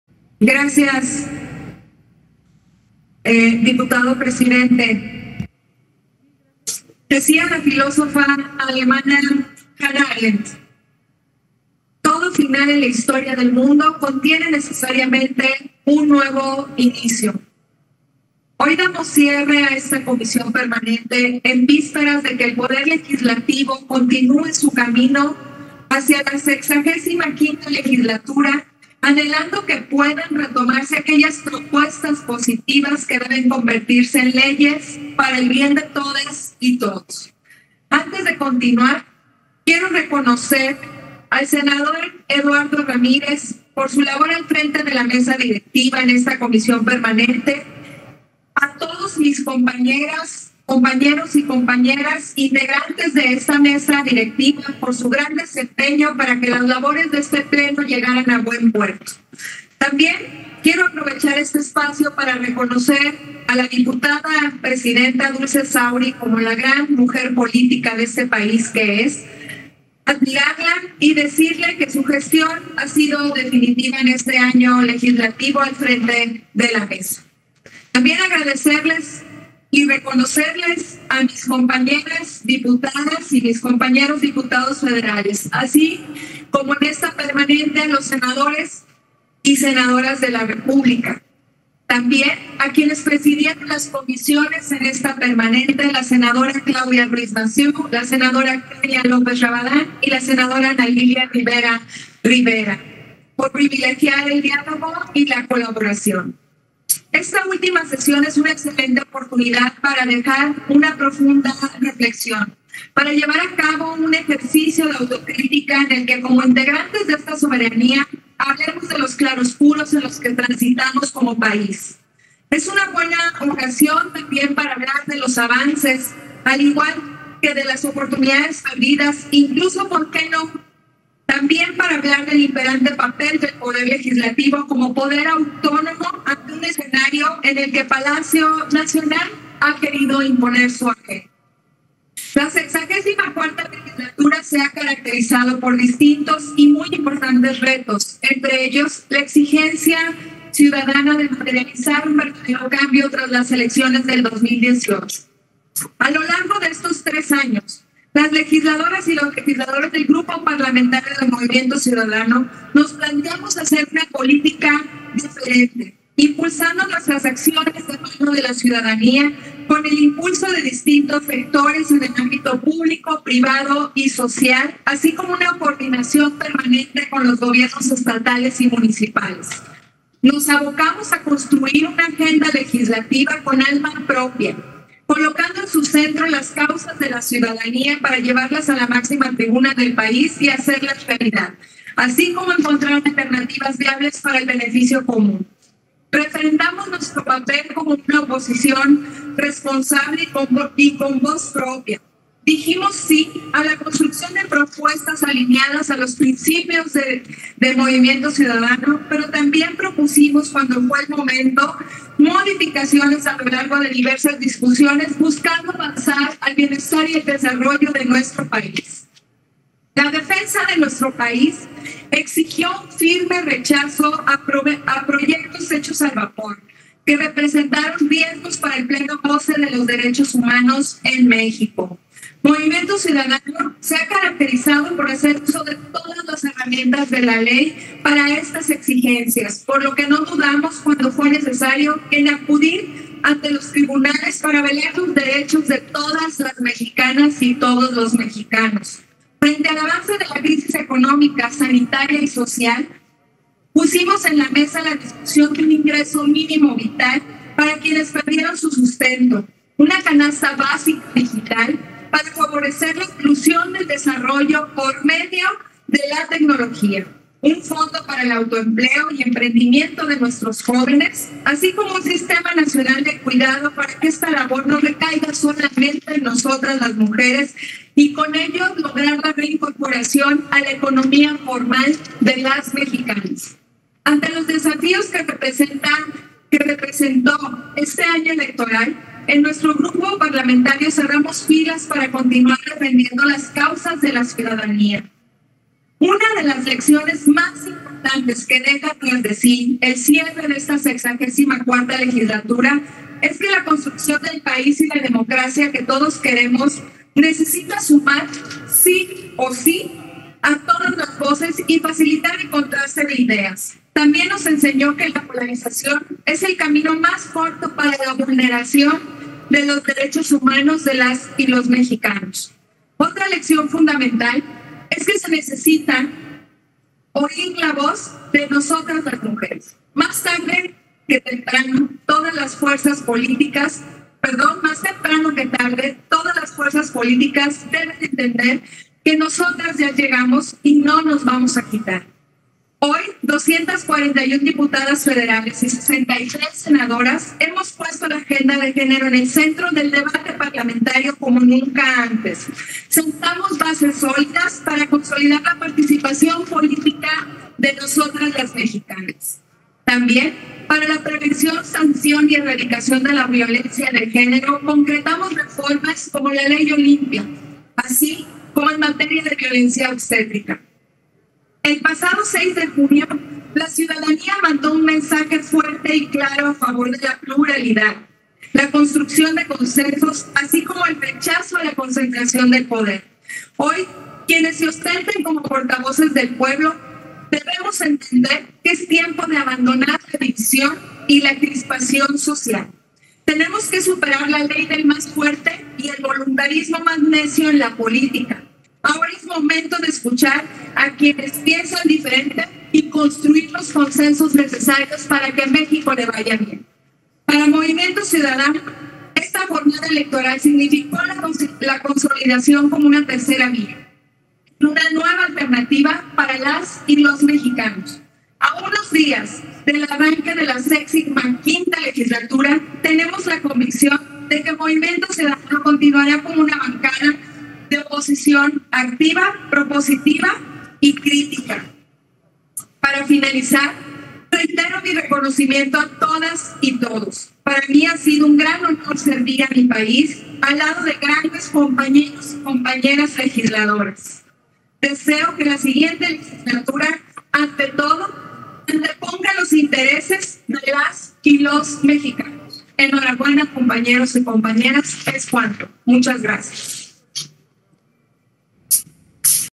Cierre de trabajos de la Comisión Permanente Coord. Dip. Fabiola Loya
En sesión de la Comisión Permanente, nuestra coordinadora Fabiola Loya reconoció el trabajo de las y los integrantes de la Mesa Directiva, así como los resultados legislativos de la actual legislatura. Asimismo, destacó la importancia de los contrapesos entre los poderes públicos, así como la necesidad de los bloques opositores en los parlamentos con el fin de crear consensos y enriquecer al parlamentarismo.